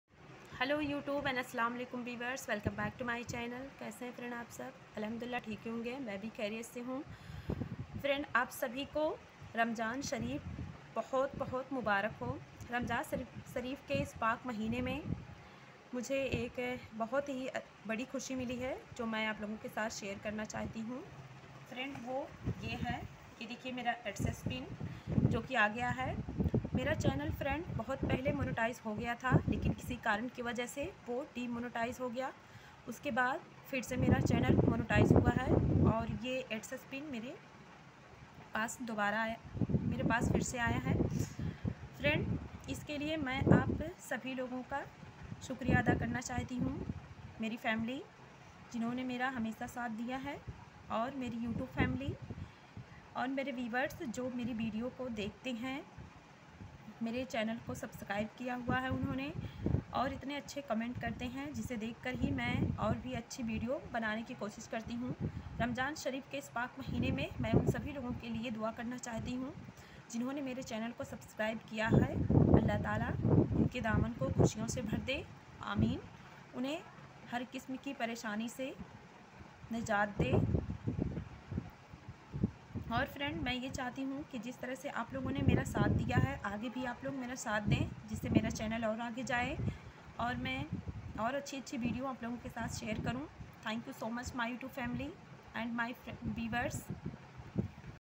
Mera Adsens pin agya hai thankyou so much my youtube family & my wiwers my all friends thankyou
Assalamualikum my youtube family today i am so happy thankyou so much my youtube family & my wiwers thankyou so much